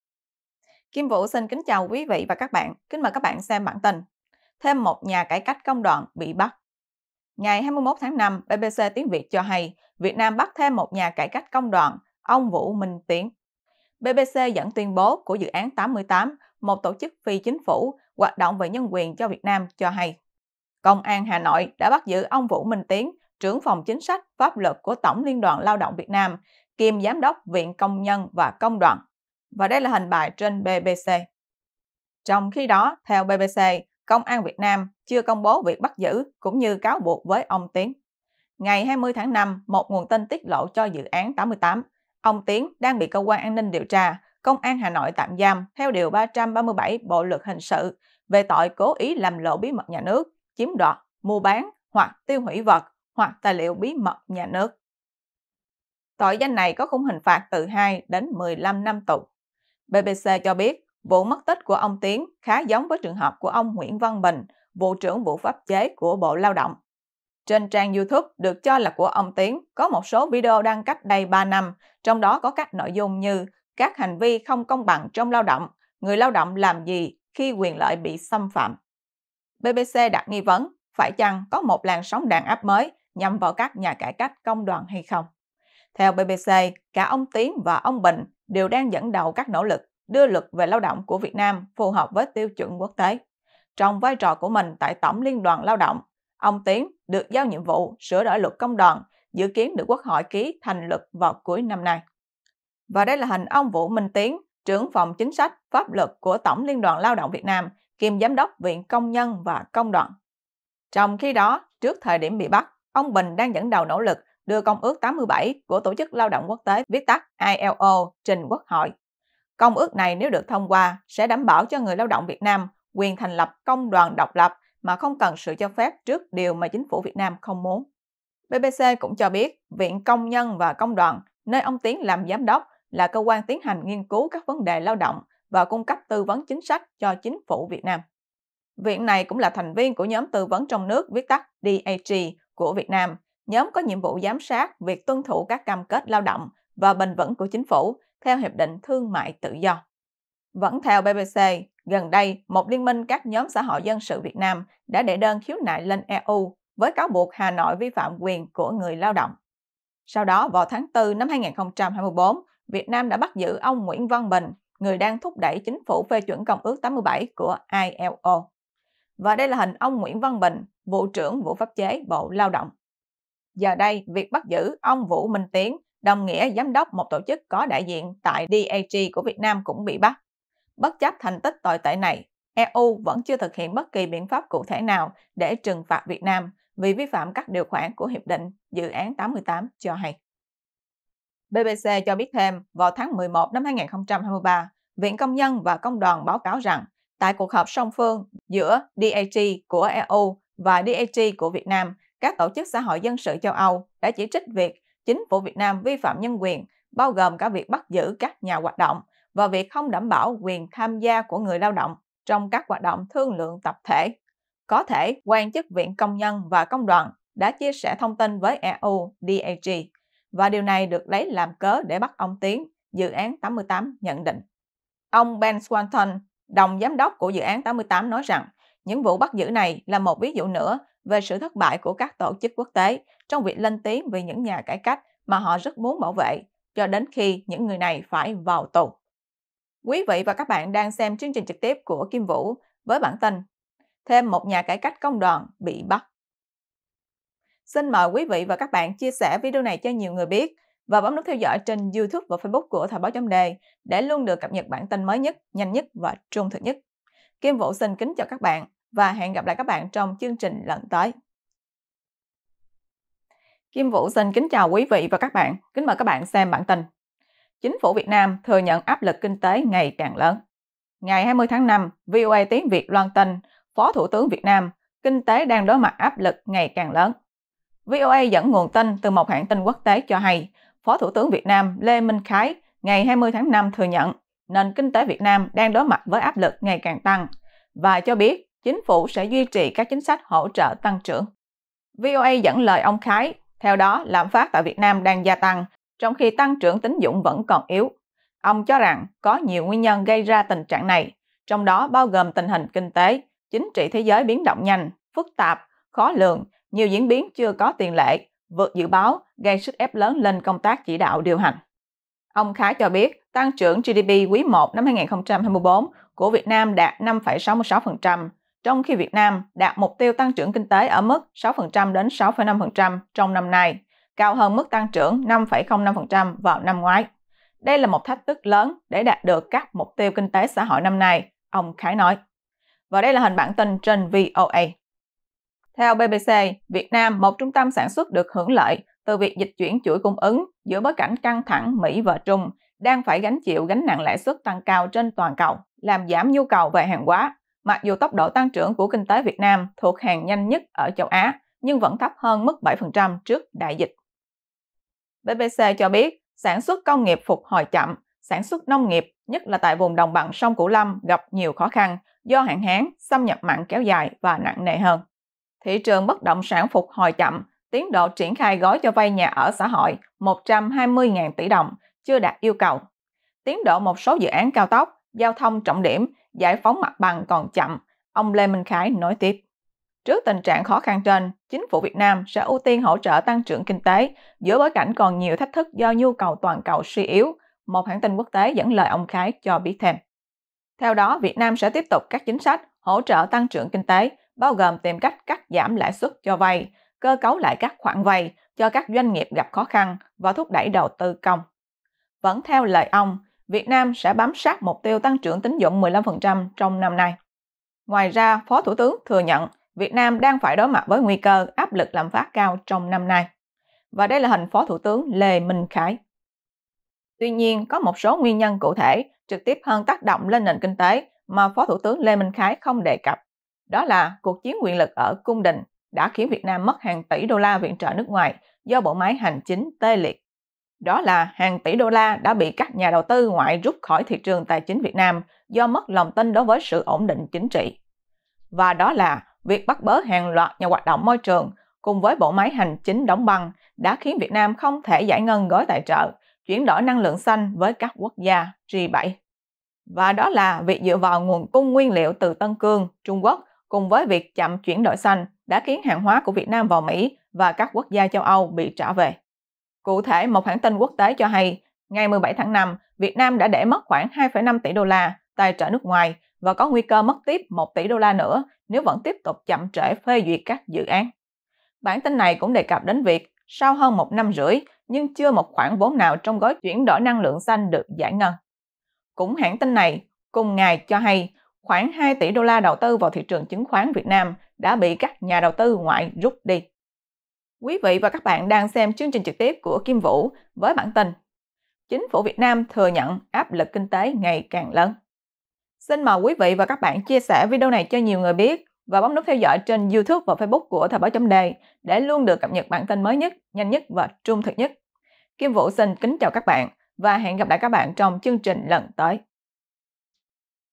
VN: Thêm một nhà cải cách công đoàn bị hốt
Dự án 88 – một tổ chức phi chính phủ hoạt động về nhân quyền cho Việt Nam, mới đây cho hay: Công an Hà Nội đã bắt giữ ông Vũ Minh Tiến - Trưởng phòng Chính sách, Pháp luật của Tổng Liên đoàn Lao động Việt Nam, kiêm Giám đốc Viện Công nhân và Công đoàn. Đây là vụ bắt bớ nhắm vào các nhà cải cách công đoàn Việt Nam, sau vụ bắt ông Nguyễn Văn Bình.